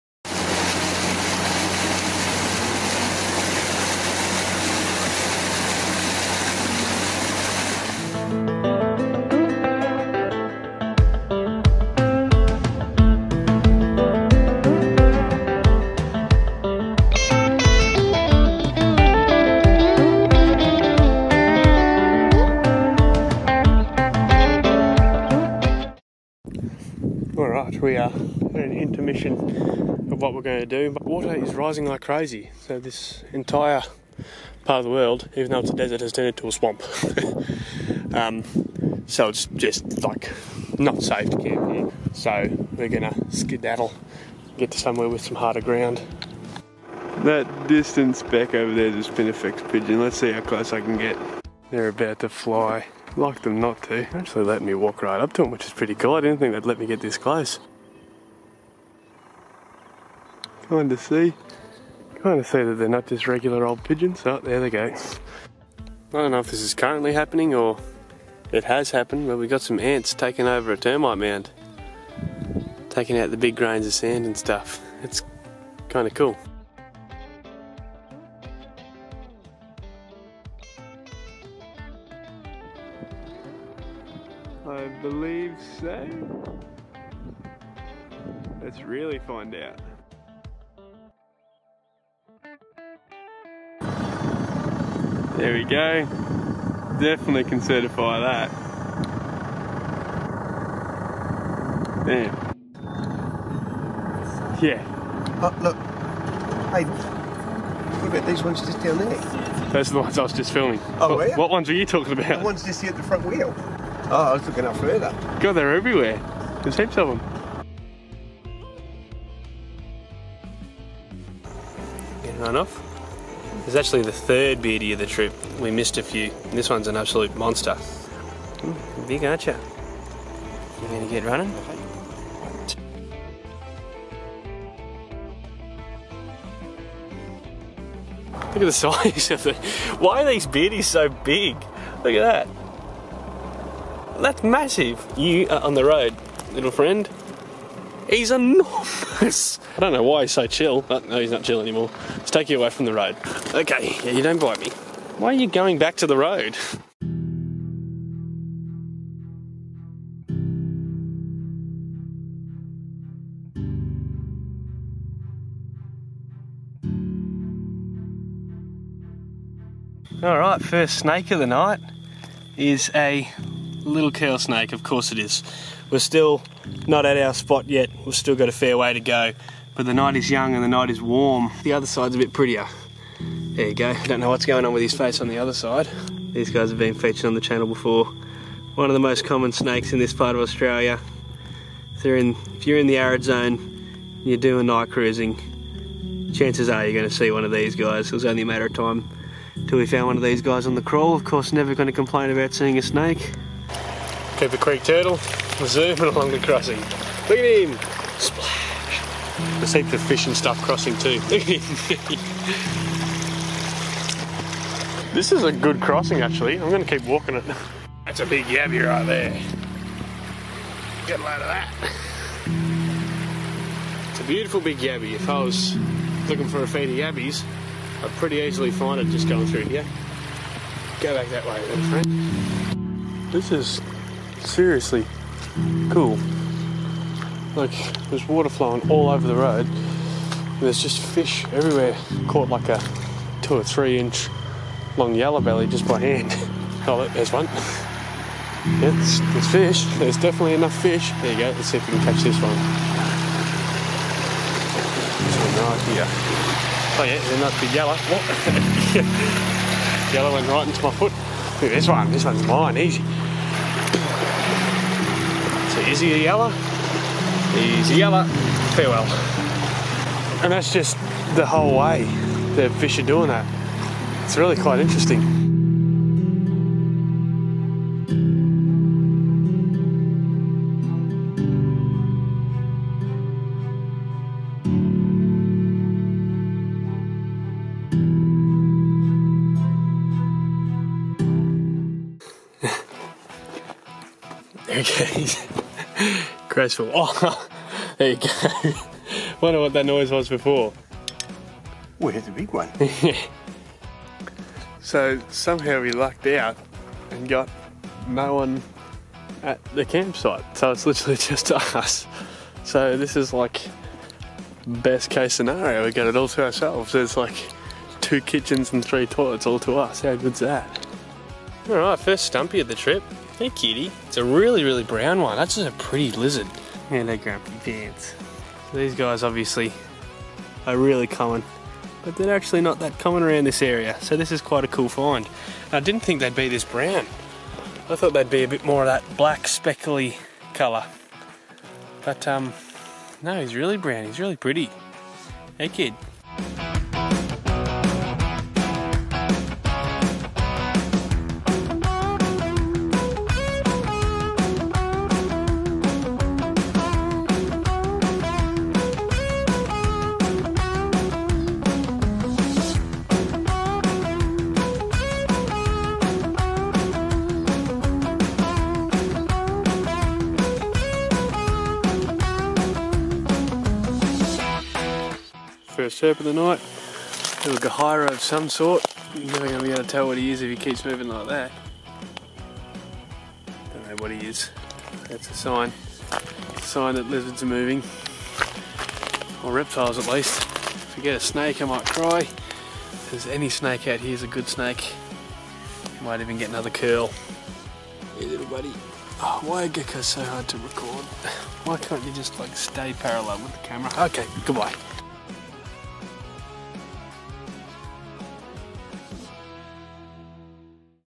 Curl Snake and BIG Beardies! | Late Season Herping in South West QLD
Episode 2 of 3 for this little mini adventure we went on, the wait for episode 3 won't be too long because it's already edited and so's the upload after that. Look at me being on top of things! As you can see in this video we got kicked out of our QLD spot because things were getting a bit moist so we had to retreat to the coldness of NSW, that being said we didn't do half bad given the circumstances!